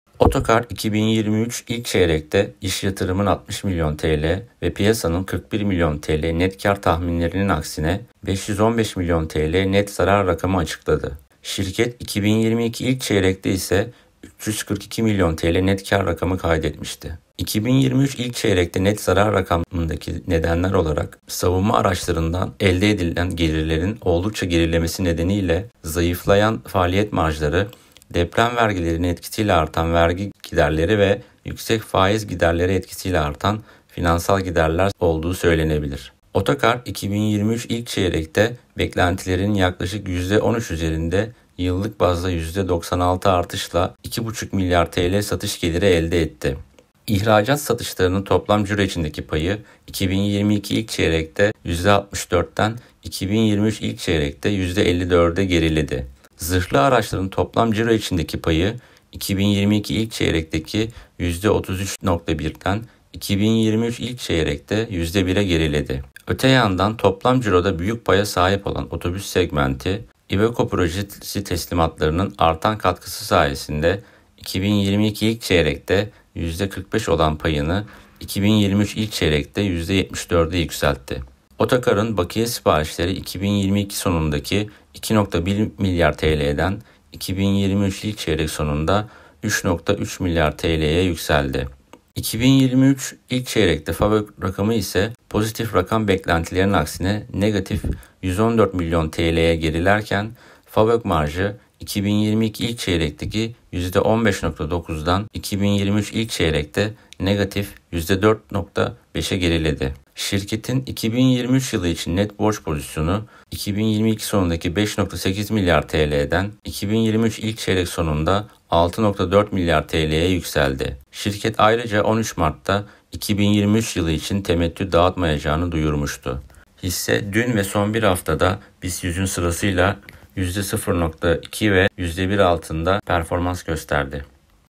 OTKAR OTOKAR 2023 1. ÇEYREK BİLANÇO ANALİZİ
📌İş Yatırım Araştırma Müdürlüğünden İlker Özgüder, Otokar'ın 2023 1. Çeyrek Bilanço verilerini analiz ediyor.